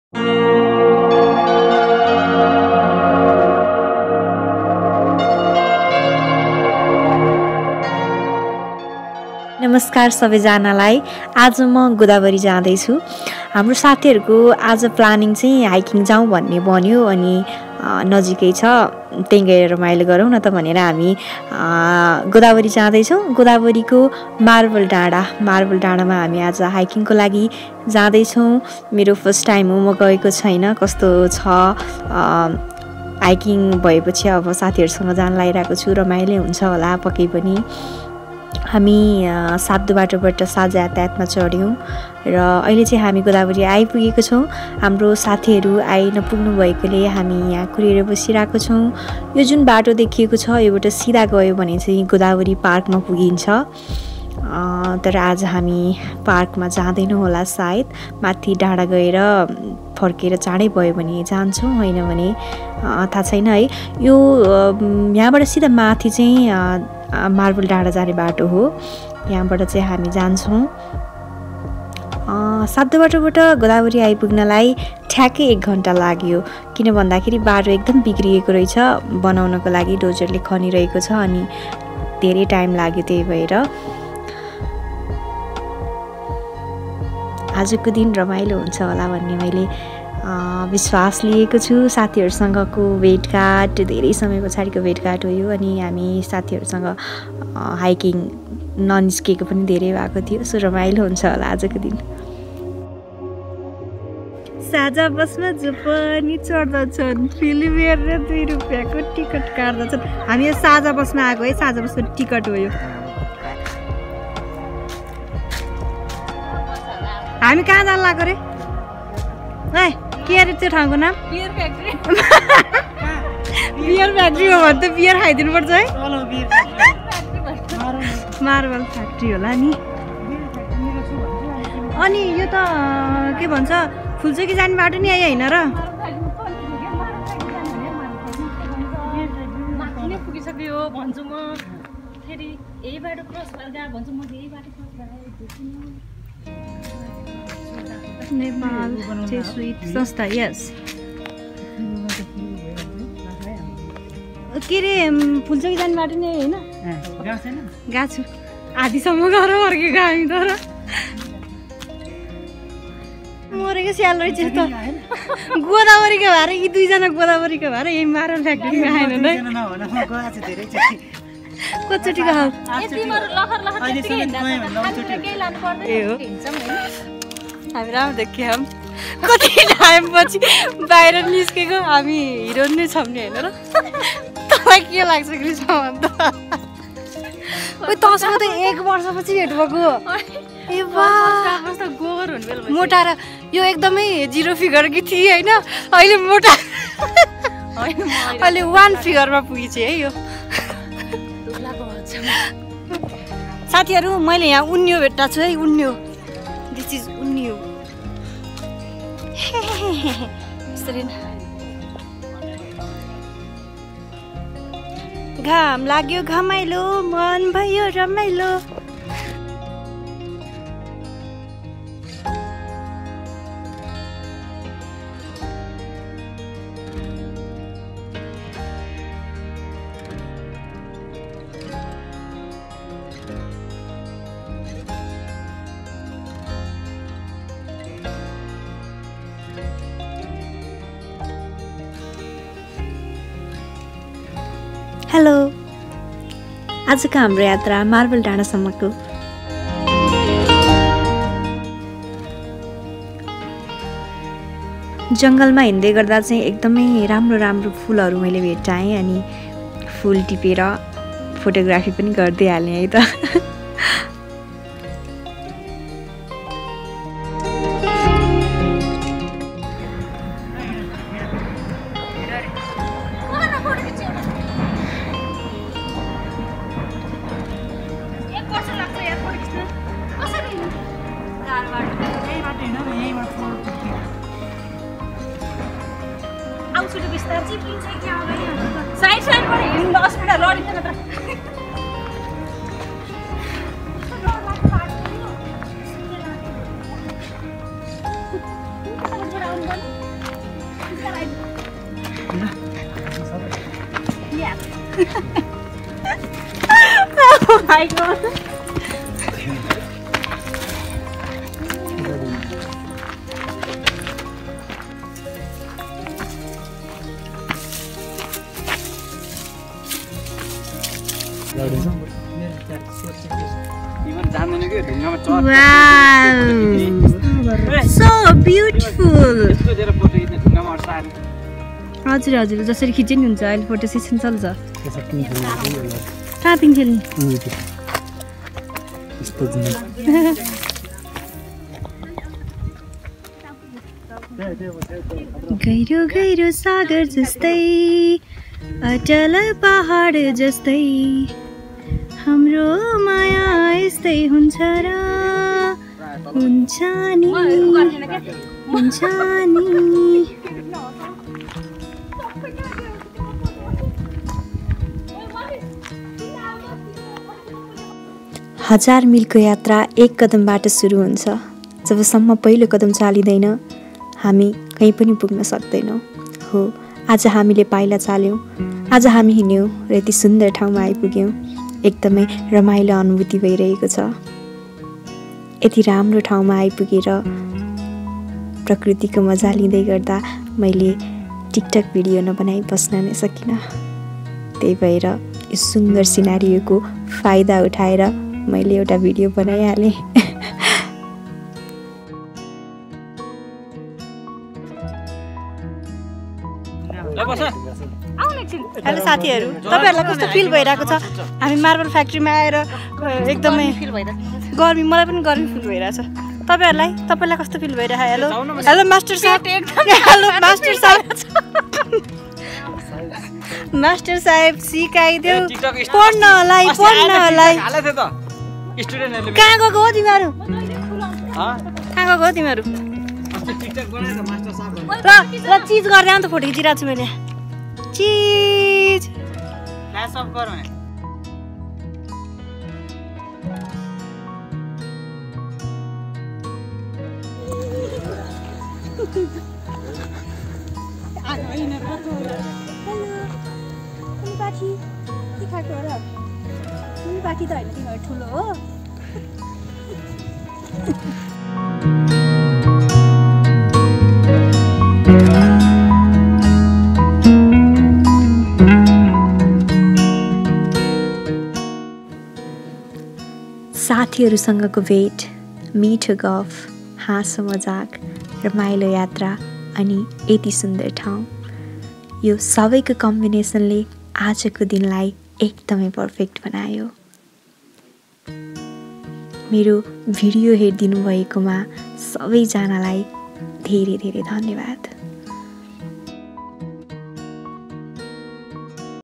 Marble Dada || Marble Factory || Godawari || Evergreen Ashmita || Travel Vlog
Marble Dada is one of the best hiking destination inside the kathmandu valley. Easy route with mesmerizing surroundings. You can explore mountains hills, forest with beautiful view of Pulchoki dada.

Marble dada is located at  Godawari, Lalitpur. It takes around 30-45 minutes by walking  to reach marble dada from Godawari bus stop.

Perfect location for short trip with family and friends.

Marble Dada
Marble Factory
Evergreen Ashmita
Promote Local Tourism
Nepal